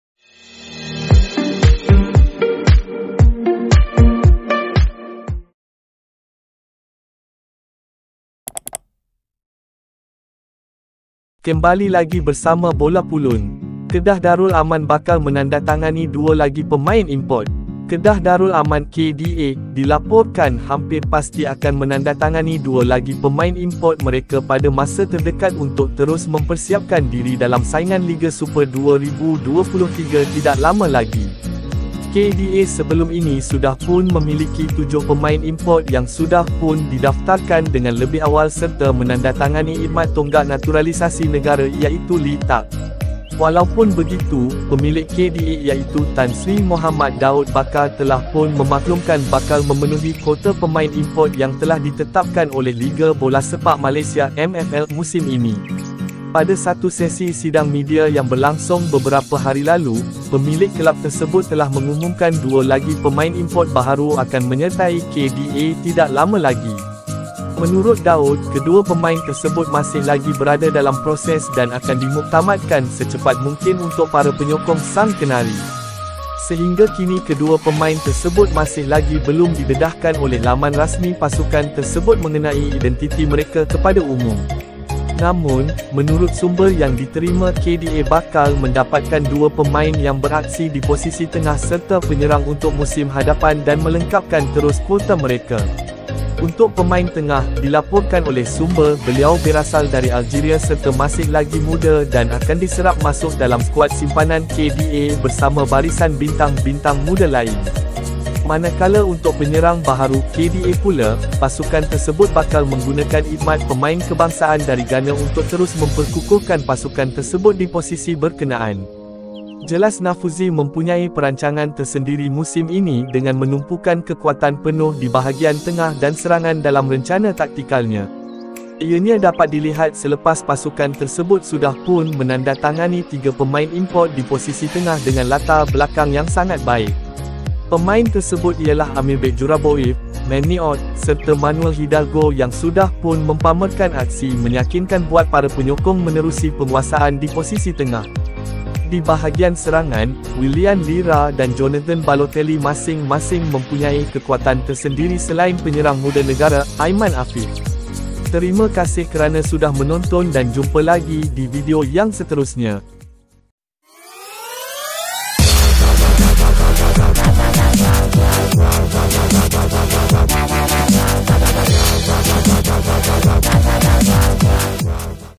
Kedah Darul Aman Bakal Menandatangani Dua Lagi Pemain Import | Bola Pulun
Kedah Darul Aman Bakal Menandatangani Dua Lagi Pemain Import